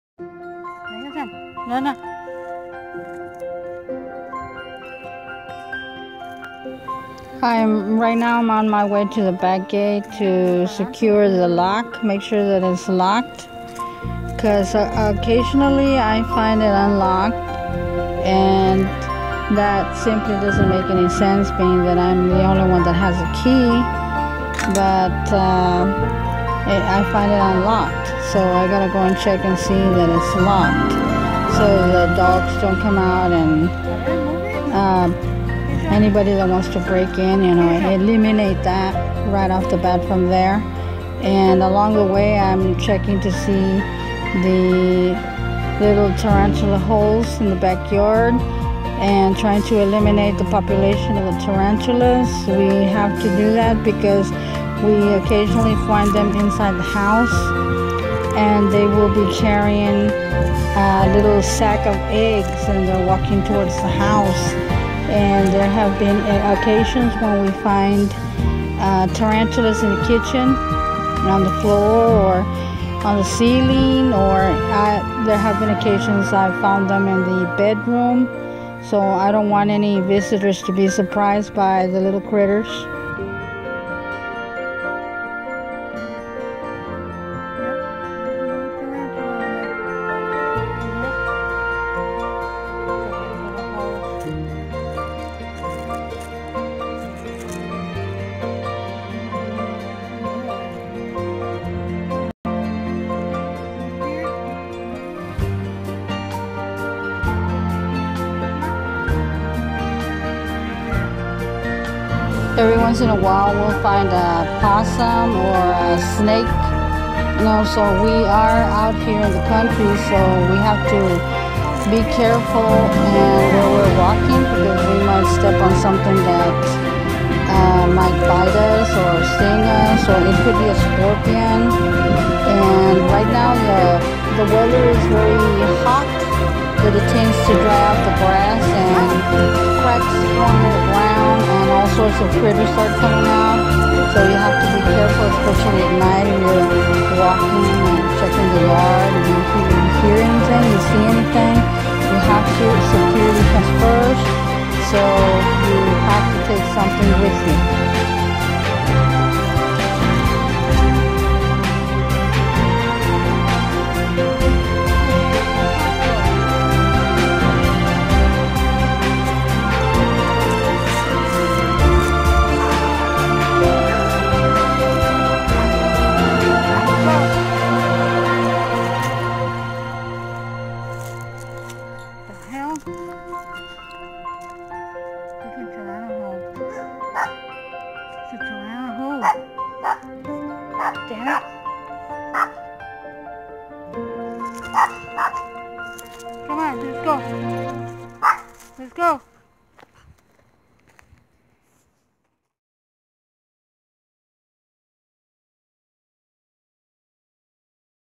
Tarantula Hunting in Texas
Downsizing the tarantula population for the safety of the family and pets.